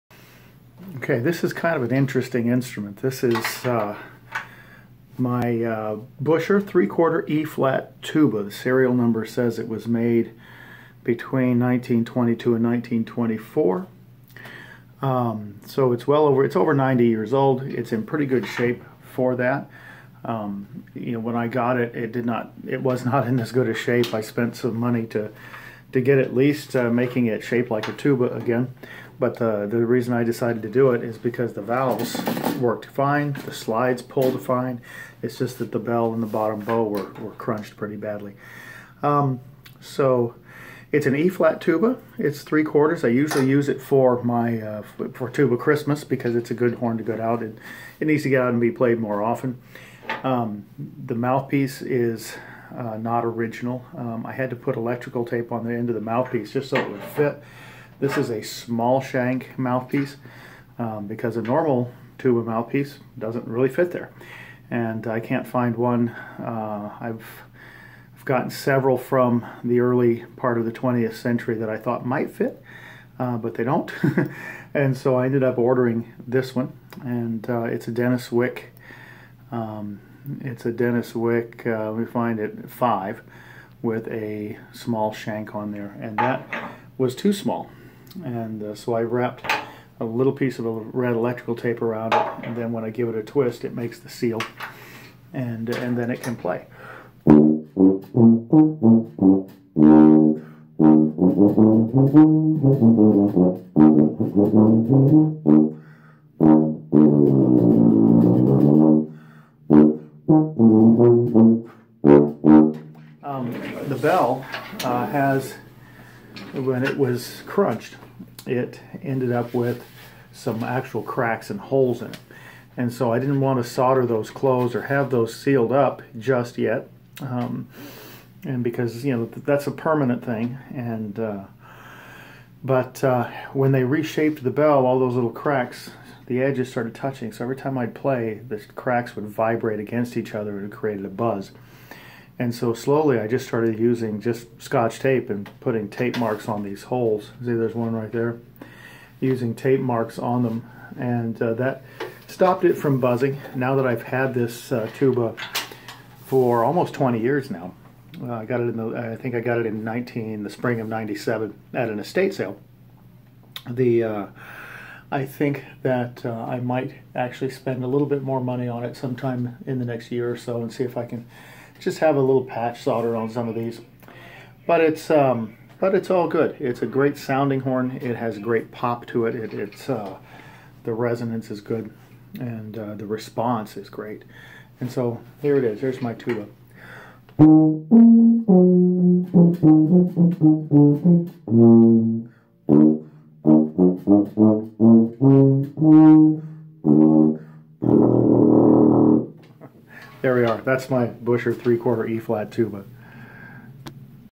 Tuba - My Buescher Eb Tuba from the 1920s
After doing some more research, it is possible that this tuba was made as early as 1921.  It doesn't make that much difference, but I like to be accurate.  Cheers.